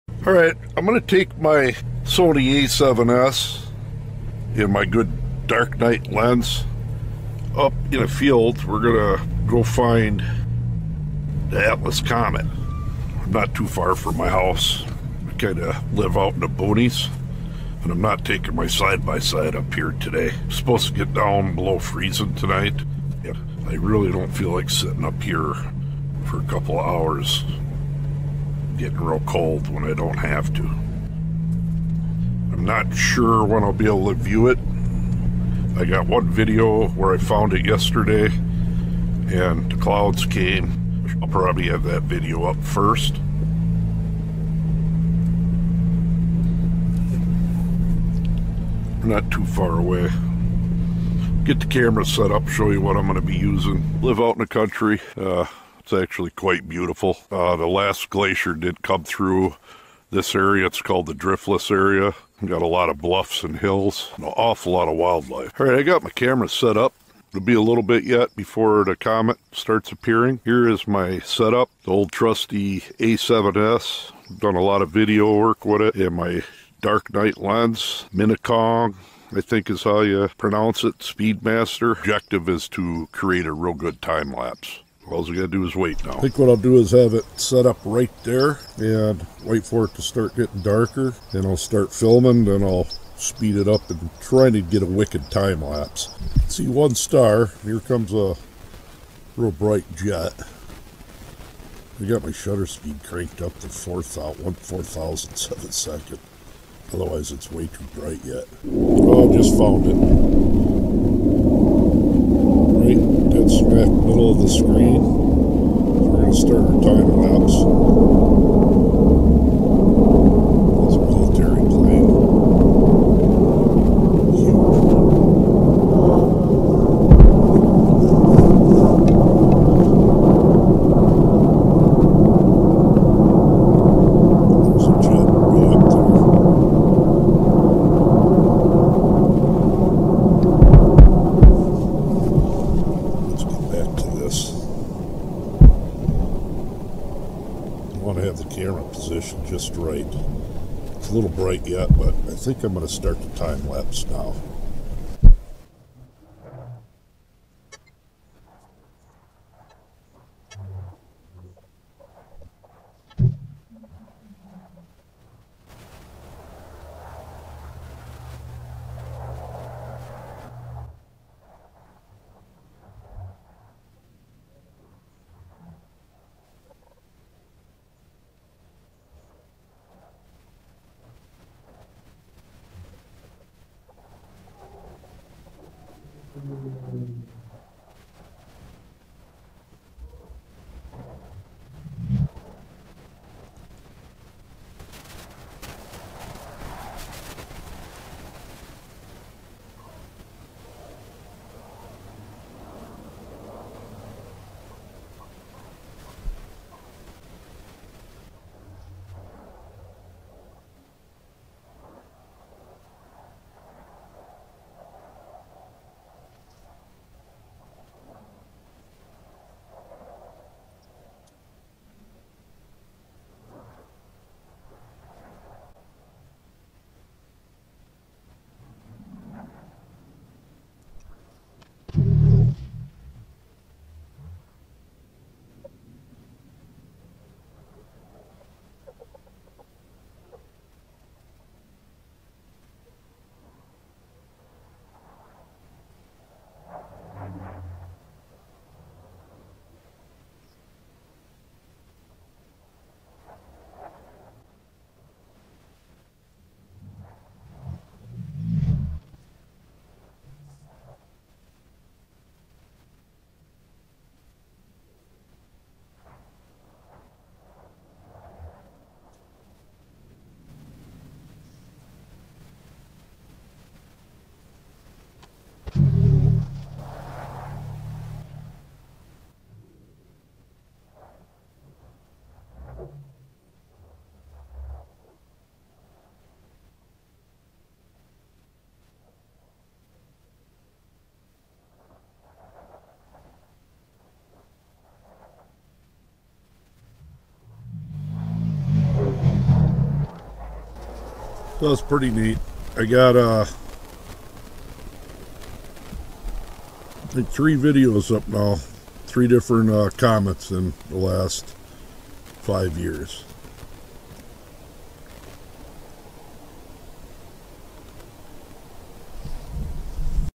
Video and Time lapse of Comet Atlas A3 along with other ojects flying around
Just used my A7s to film the comet on the 2nd night.The 14th of Oct
Moon was washing a lot out so I could not crank the A7s up.
But still was nice and peacefull